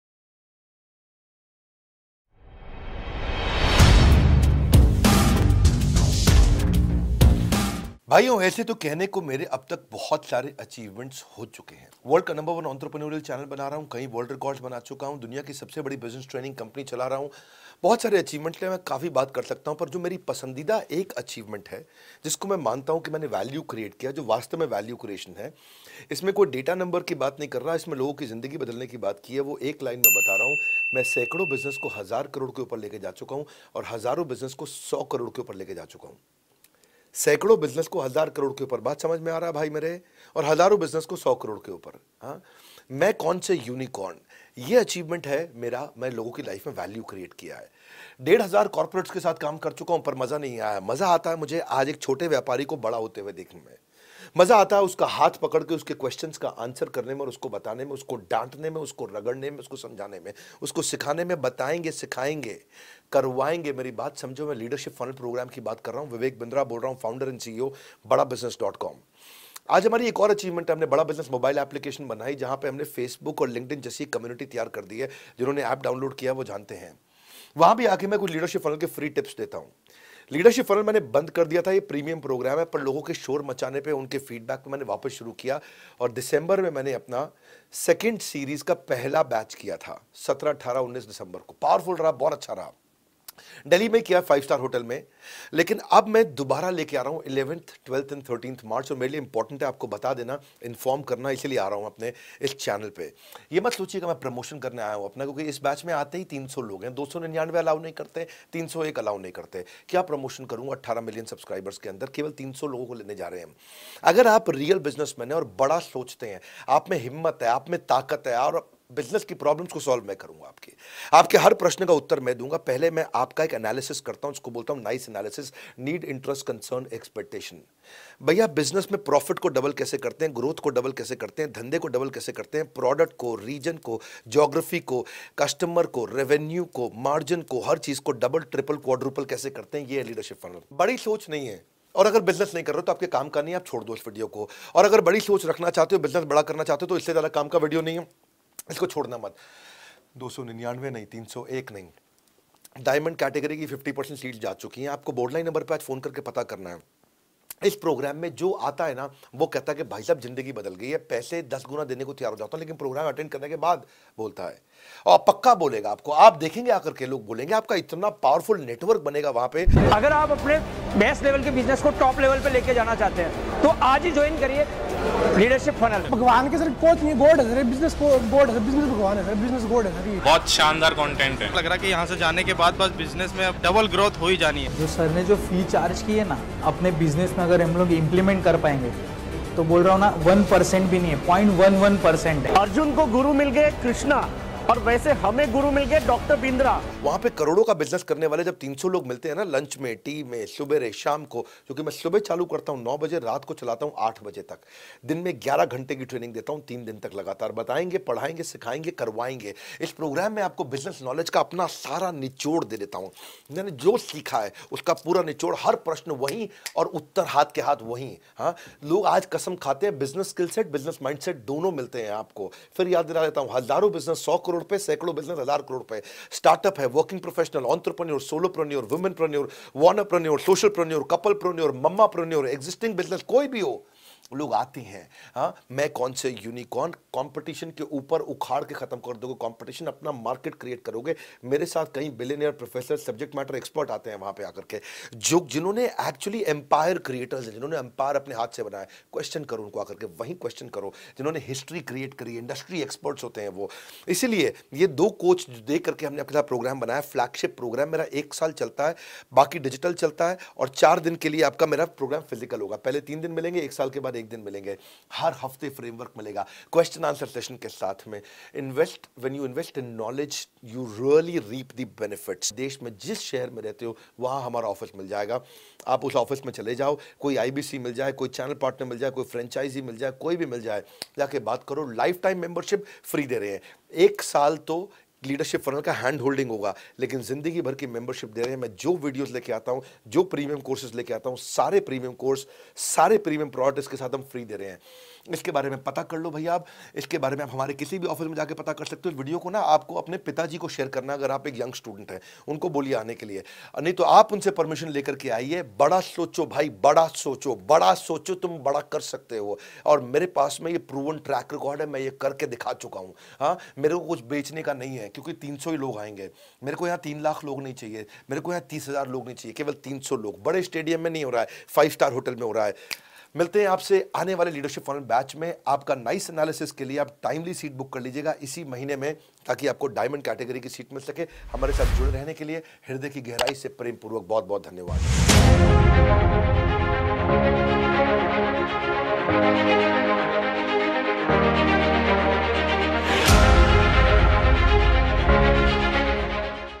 Leadership Funnel Program क्या होता है?
Leadership Funnel Program क्या होता है? और यहा Vivek Bindra sir ऐसा क्या सिखाते है की इस प्रोग्राम की कसमें कहते हैँ ?

Get A Chance Free Consulting Bada Business & team "Vivek Bindra"
Solve Your Business Problem & help to grow your business next level